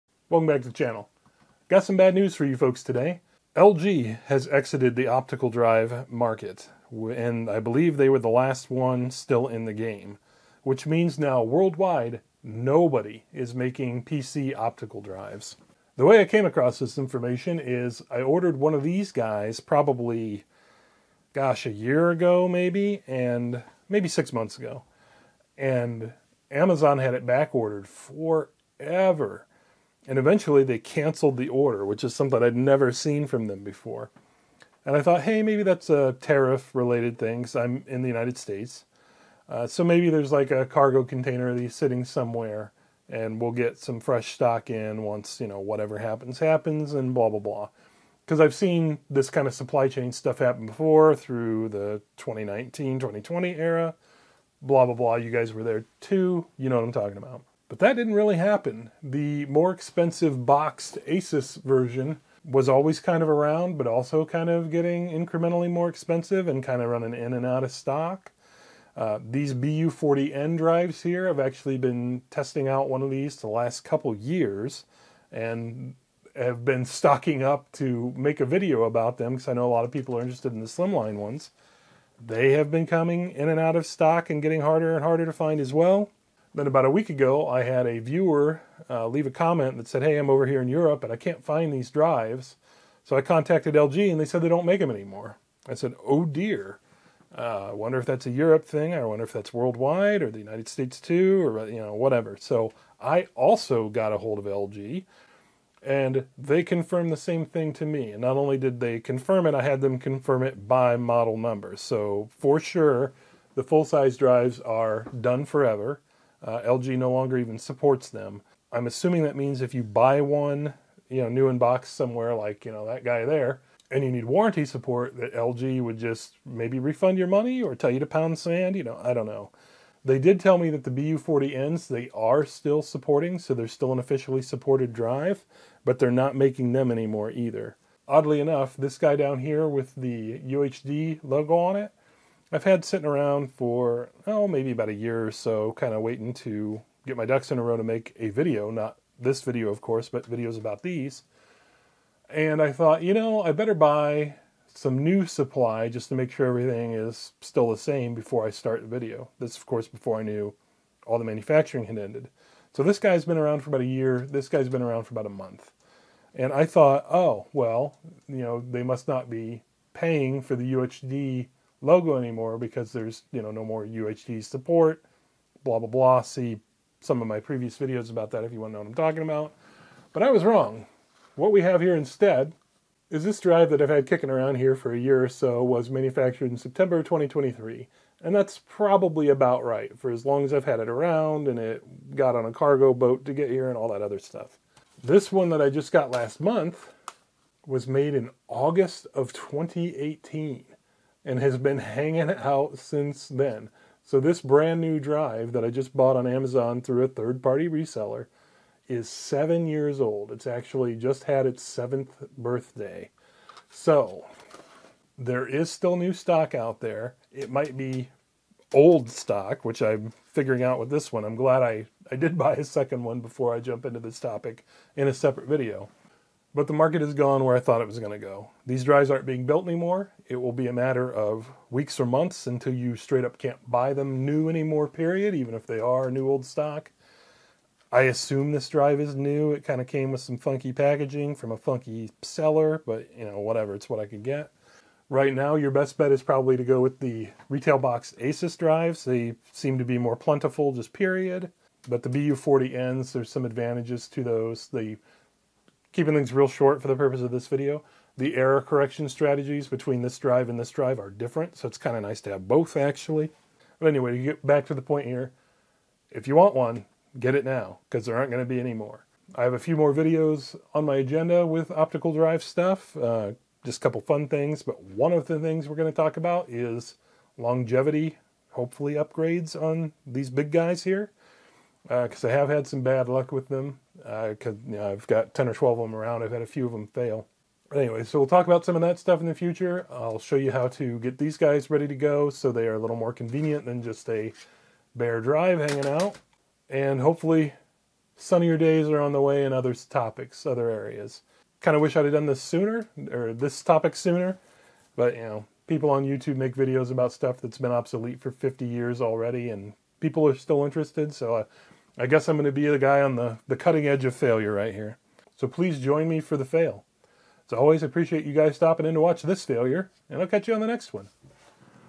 PC Optical Drives Are No Longer Being Made
We have officially entered the end of the lifecycle of optical media. No new CD-ROM, DVD-ROM, Blu-ray, etc drives are likely to ever be built again for PC applications.

Asus Drive
LG Drive
LG Drive 2
Full-Size External Enclosure
Slim Drive

Send me everything... except to jail:
The Broken Life
4000 E. Bristol St.